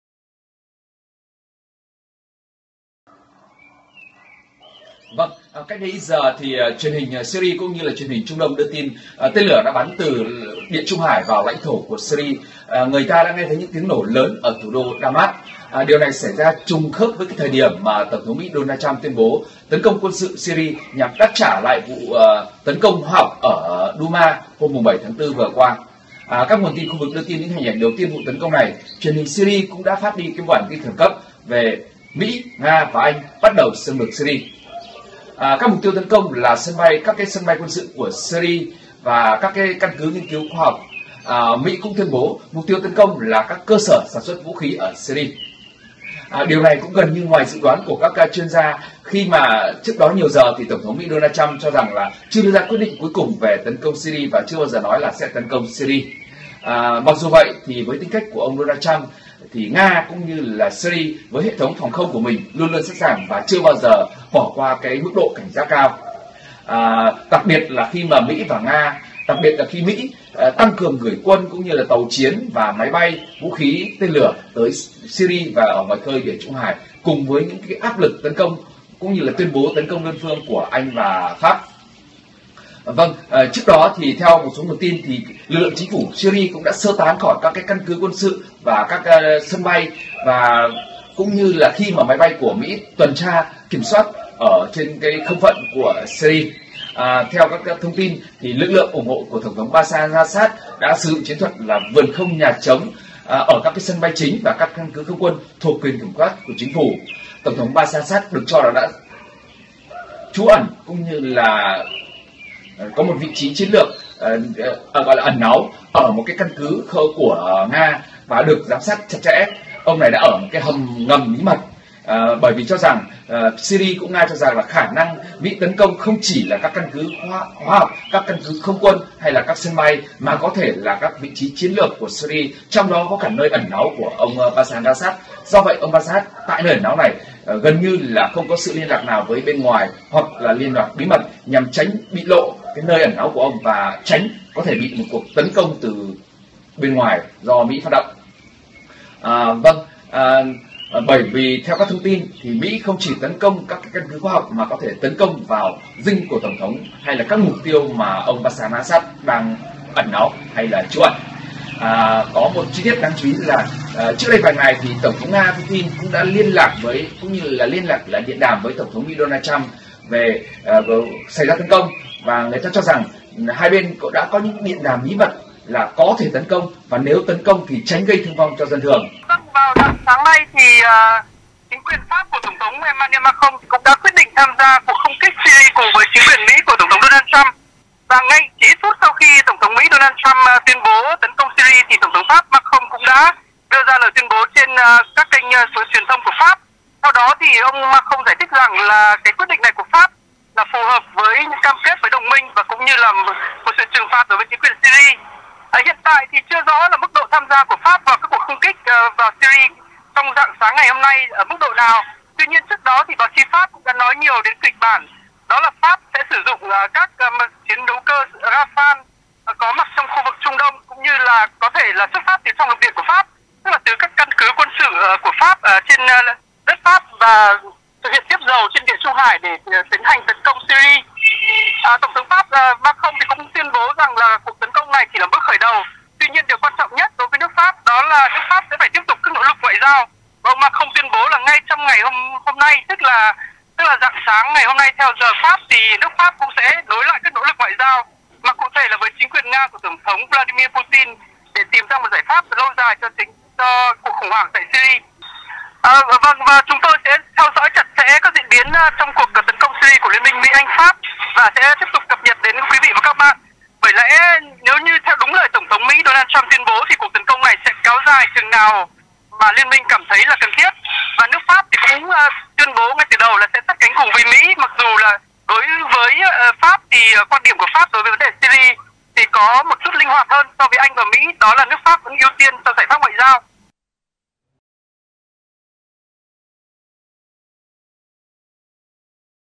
Tổng thống Mỹ ra lệnh tấn công Syria | VTC1
VTC1 | Ngày 14/4, tổng thống Mỹ chính thức hạ lệnh tấn công Syria. Thông tin mới nhất được phóng viên thường trú của VOV tại Trung Đông và Pháp.
📢 Tải ứng dụng VTC Now tại 🏷️ (Android) 🏷️ (iOS)